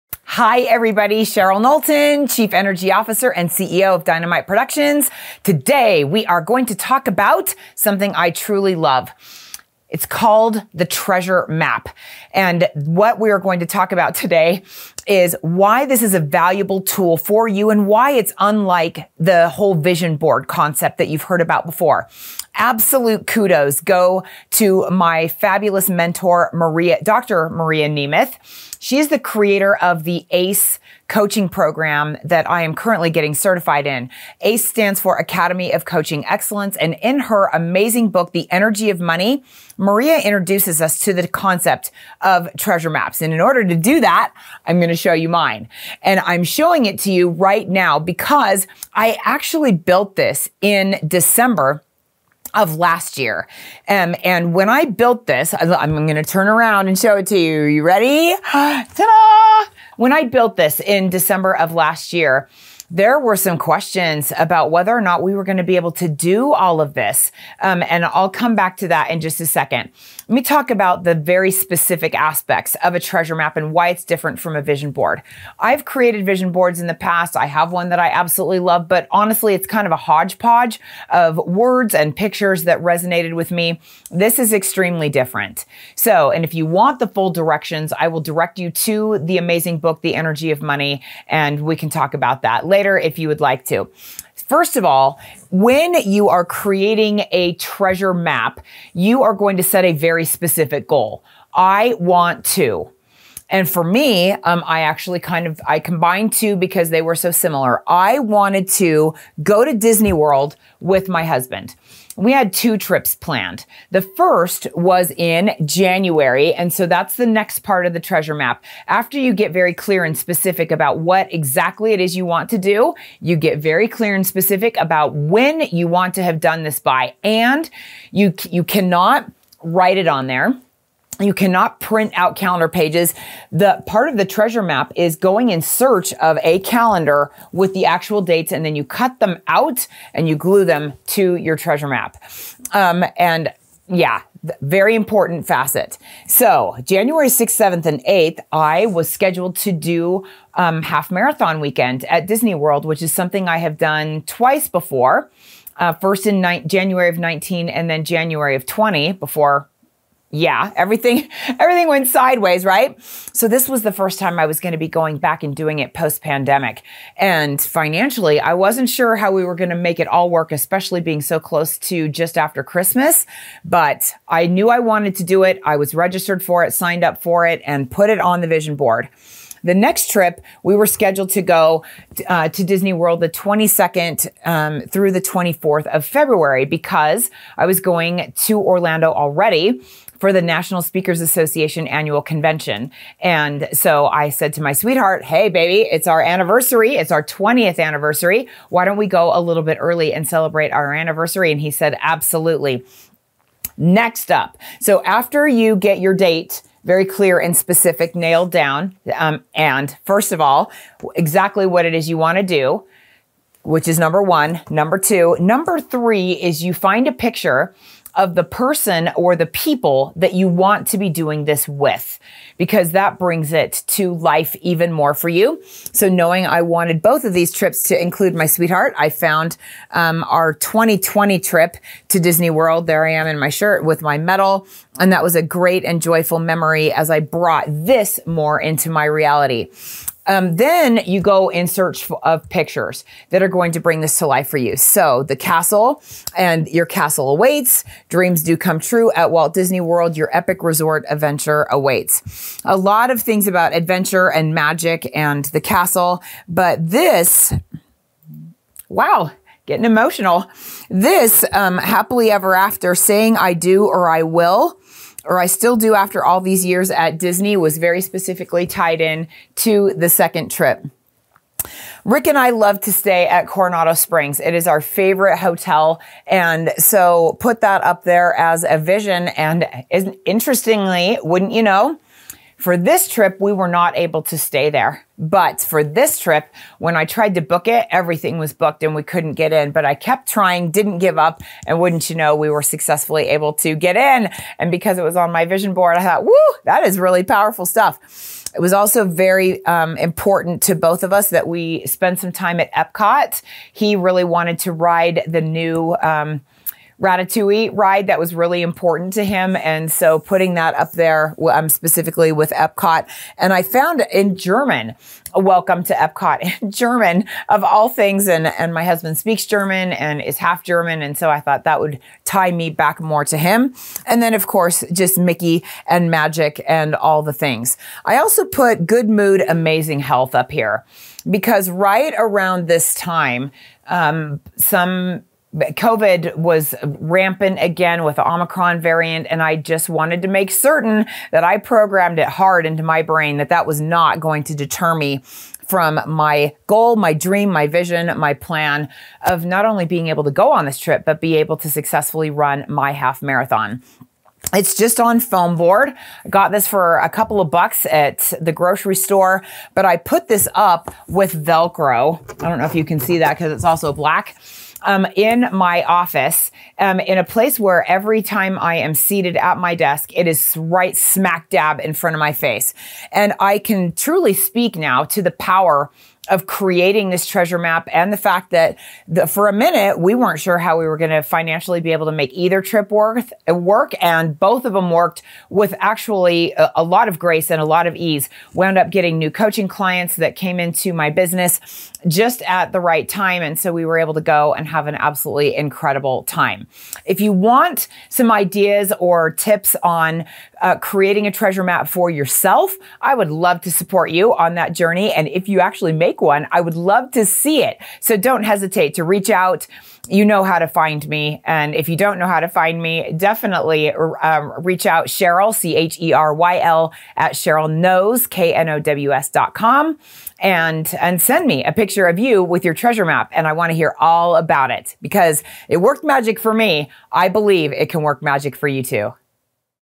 Dream Board: Vision Board - with a Twist!!
This is another take on the Vision Board but with a twist. It is the Treasure Map. If you have a specific goal.  What do you want to do. When you want to do it. Then look for images to support this vision by mapping it out on a board.